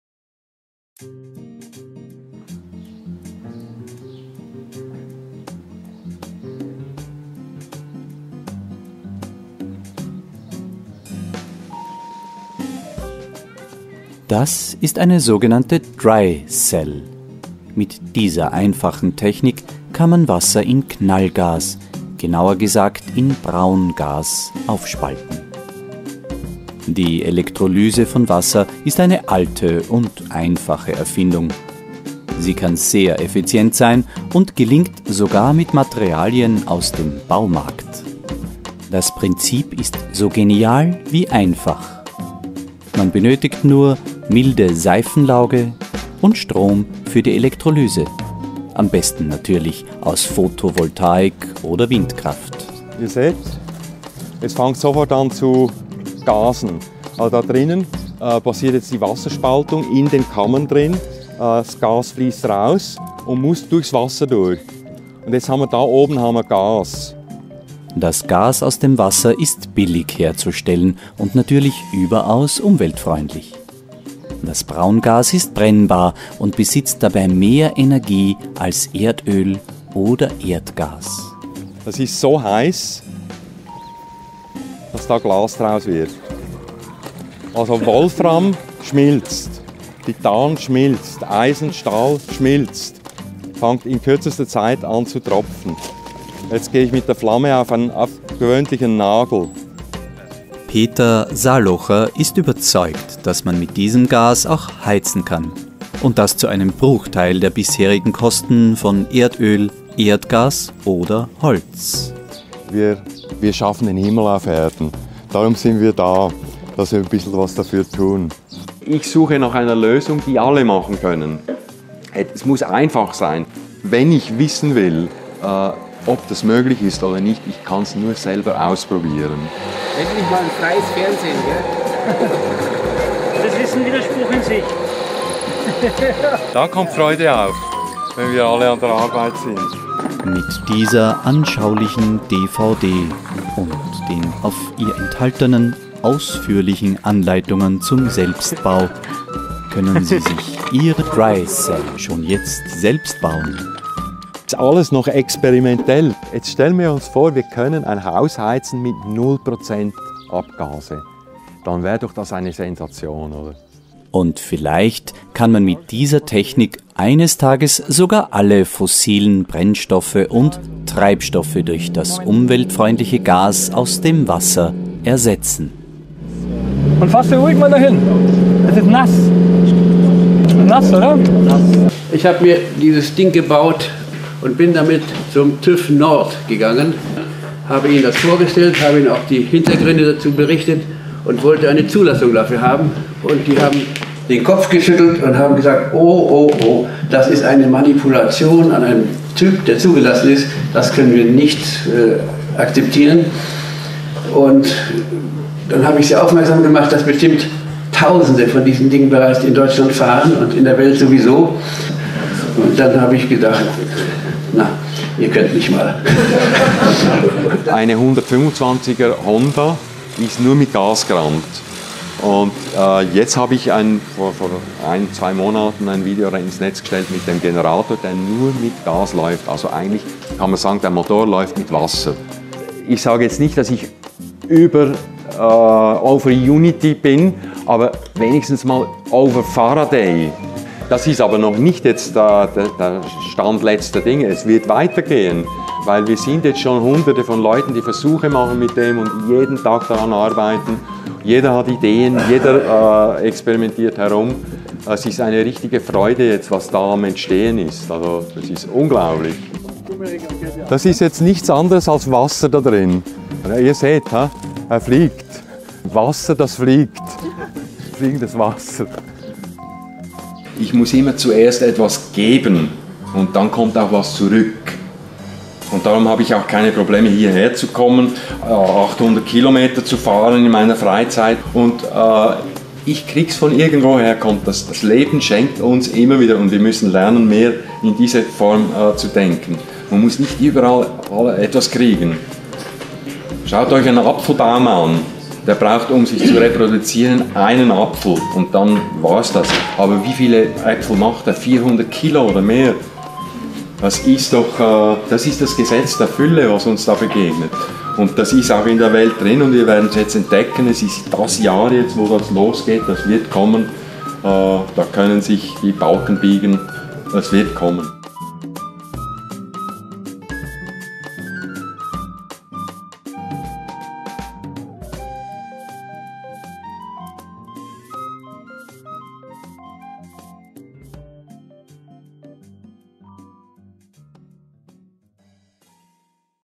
Wasser Statt Sprit - die Anleitung für alle
Autarkie für alle: Das Material gibt's im Baumarkt, die Anleitung gibt's und

Download oder DVD mit Selbstbau-Handbuch:
- 4 Stunden Spieldauer
- Schritt-für-Schritt-Anleitung
- 10 spannende Experimente
- vollfarbiges Anleitungsbuch DIN A5, 76 Seiten
- dt. oder engl. Sprache